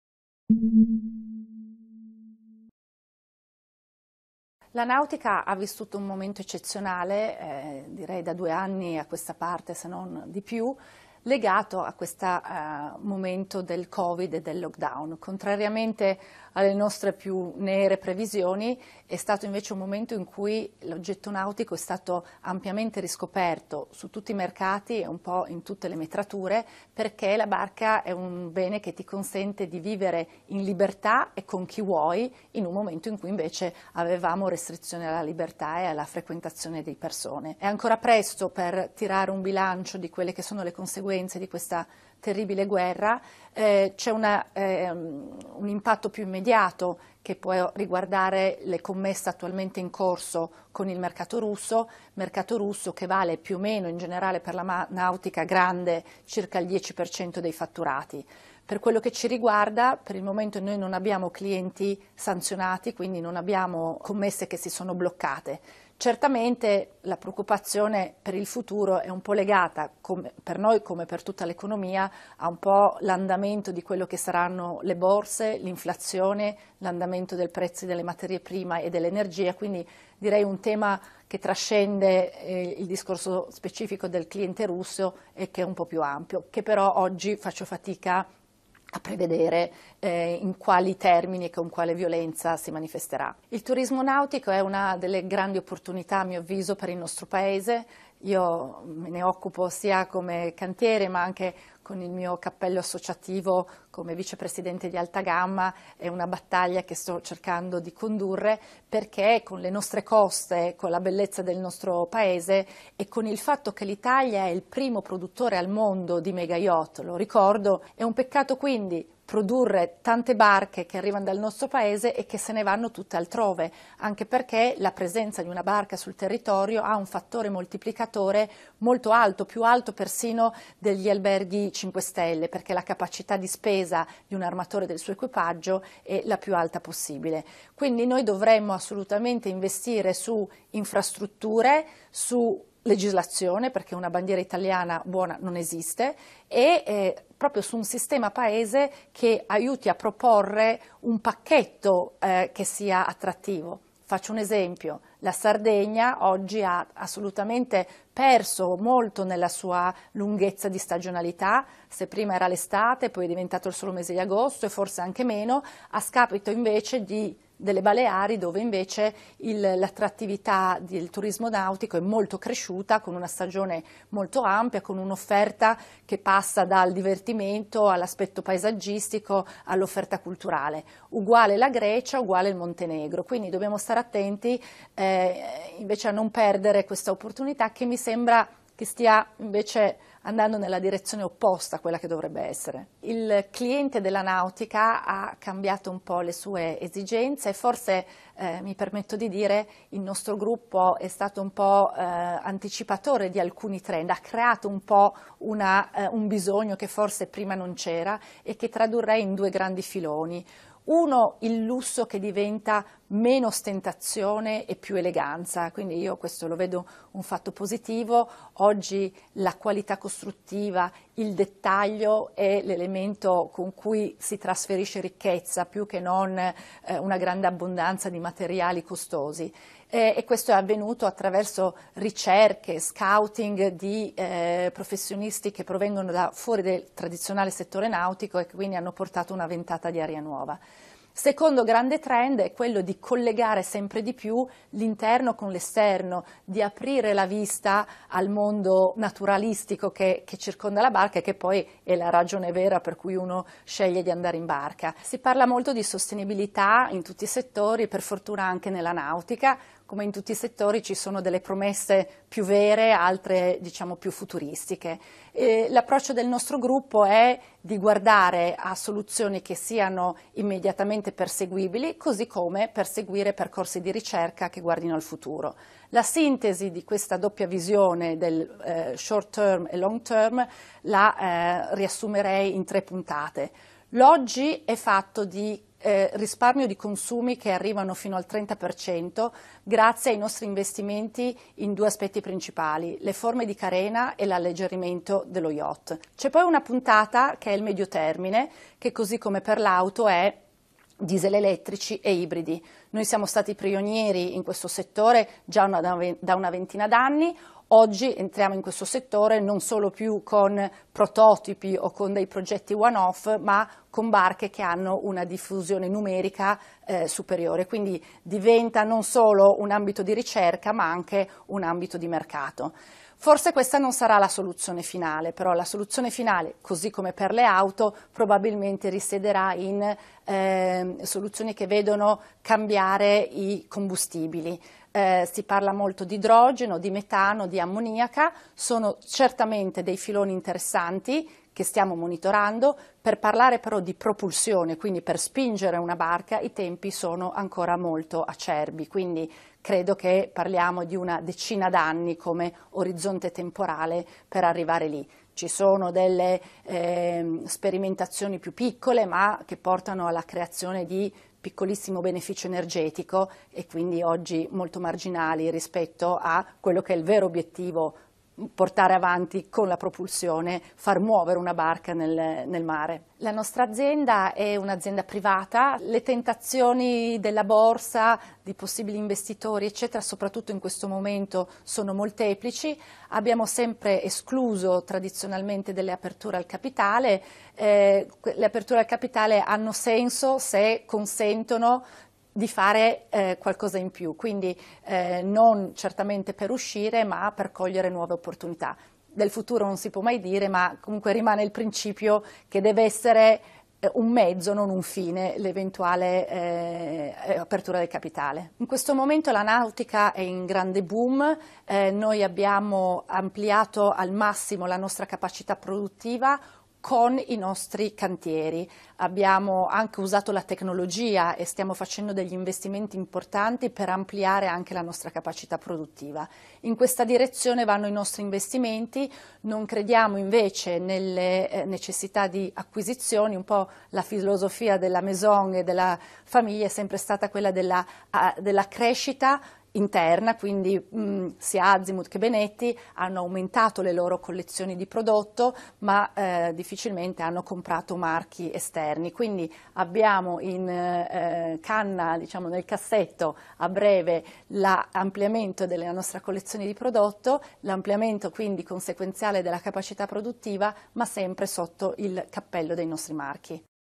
Giovanna Vitelli (Azimut Benetti): l’Italia è il maggior produttore di barche, ora punti sul...
La vice presidente del gruppo torinese: «È un grande moltiplicatore di ricchezza. Ma servono investimenti in infrastrutture, in legislazione». La Borsa? «Solo se ci sono opportunità in più». Nessuna acquisizione, ma aumento delle capacità produttive. ( di Maria Silvia Sacchi / CorriereTV ). Guarda il video su Corriere